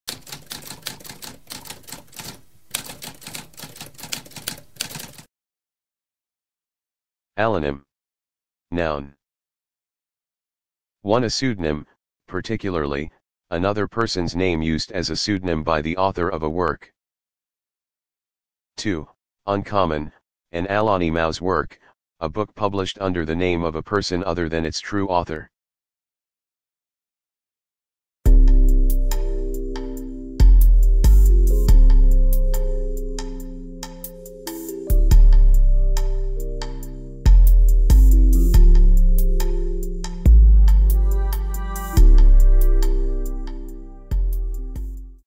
What does allonym mean?
A spoken definition of allonym.

Intro Sound:
Typewriter - Tamskp